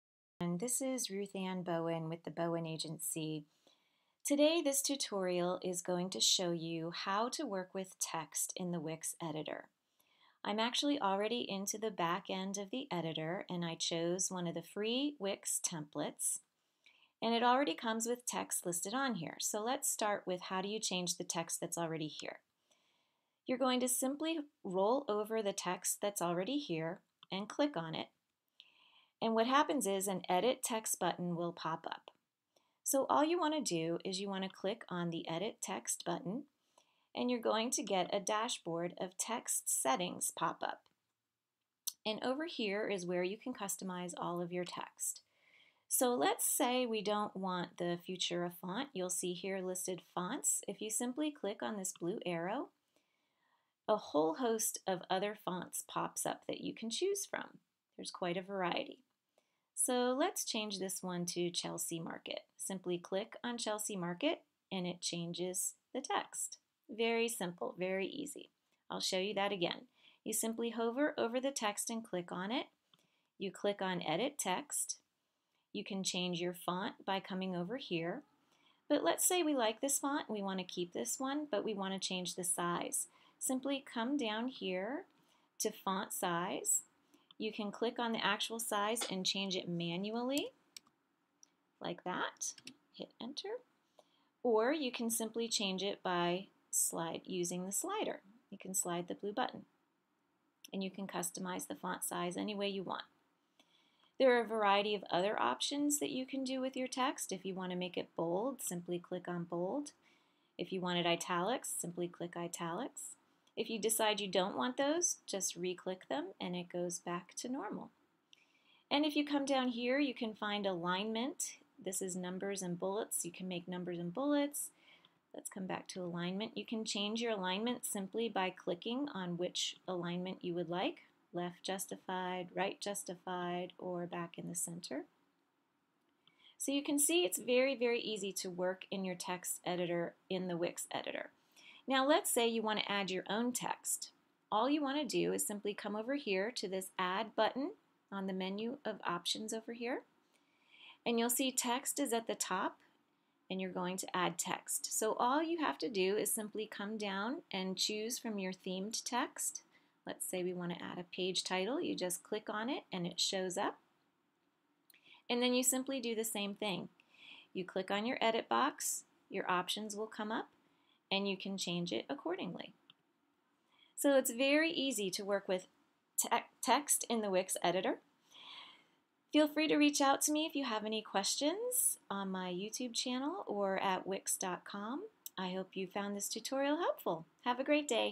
How To Use Text In Wix Editor
This is a quick how-to use Text in the Editor. Created by Ruthann Bowen.